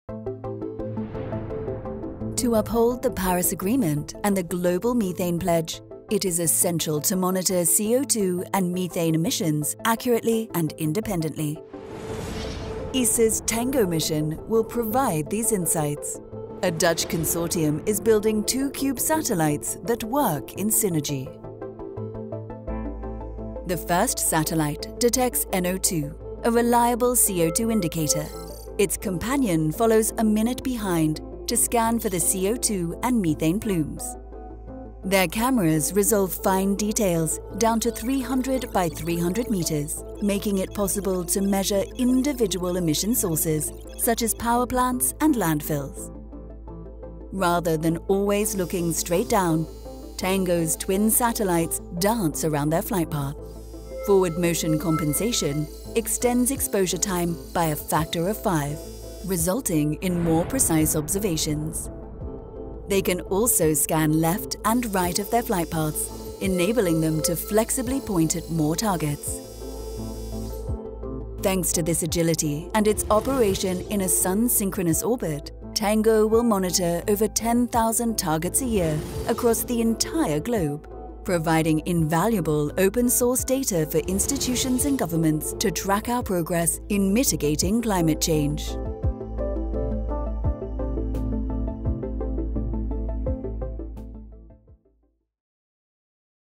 TANGO greenhouse gas satellite
The Twin Anthropogenic Greenhouse Gas Observers (TANGO) space mission will be launched into polar orbit in 2028. Two satellites will zoom in together on factories and power plants to measure their CO2 and methane emissions. TANGO can distinguish individual facilities with 300×300 meter ground pixels. Together with KNMI, SRON is the scientific lead for the mission and is co-developing the TANGO-Carbon instrument.